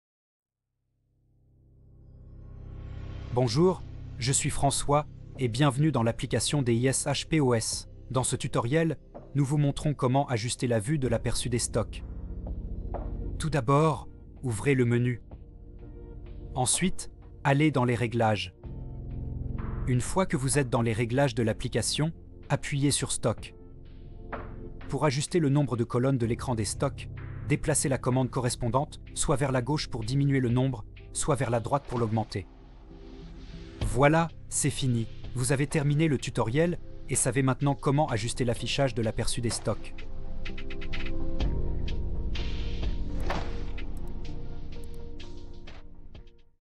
DISH POS- Comment puis-je ajuster l'affichage de l'aperçu des actions pour mon appareil ? (Tablette)
Dans cette vidéo, François va vous montrer comment ajouter un nouveau groupe de produits.

00:00 Intro
00:12 En allant dans les réglages
00:20 Écran de stock
00:34 Outro